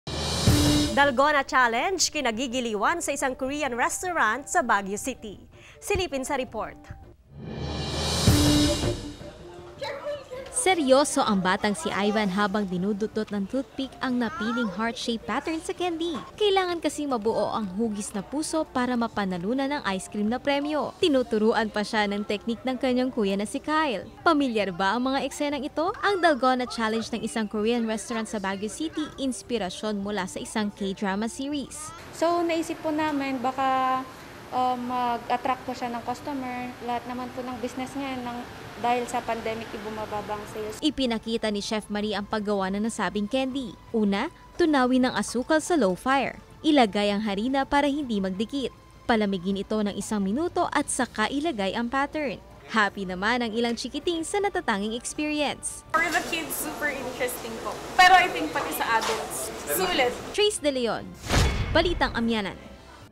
Balitang Amianan: 'Dalgona' Challenge
‘Dalgona’ challenge, kinagigiliwan sa isang Korean restaurant sa Baguio City.